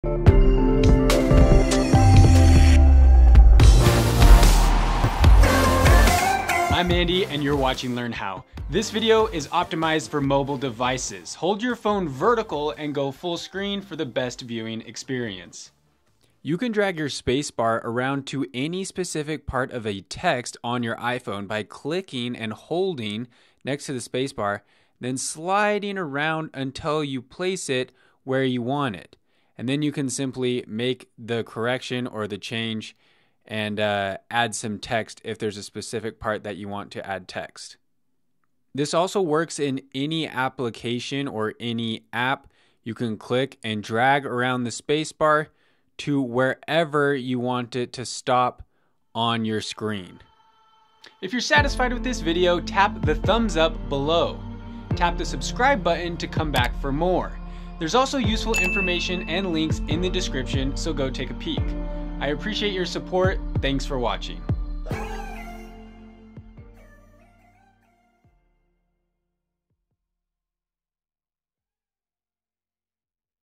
How to Move Your iPhone Spacebar Trick!
Learn how to move your spacebar around on your iPhone to the exact spot you want it.  This is a really useful trick that I recently learned.  Share it with a friend 👀